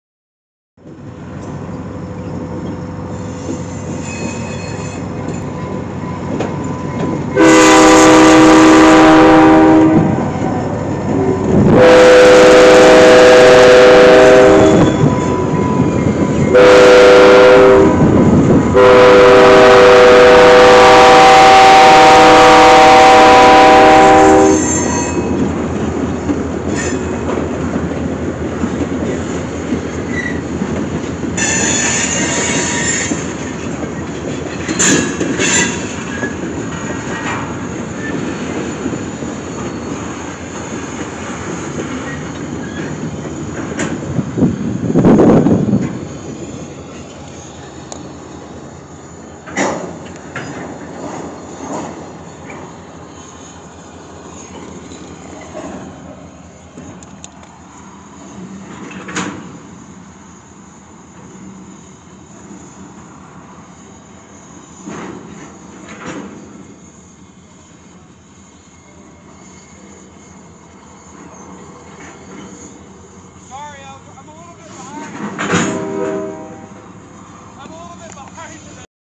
Short movement of white tanks by Portland & Western Railroad @ Whiteson, Oregon 04-08-22
Portland And Western Railroad is shown moving some white tanks near the 99W in Whiteson, Oregon !!!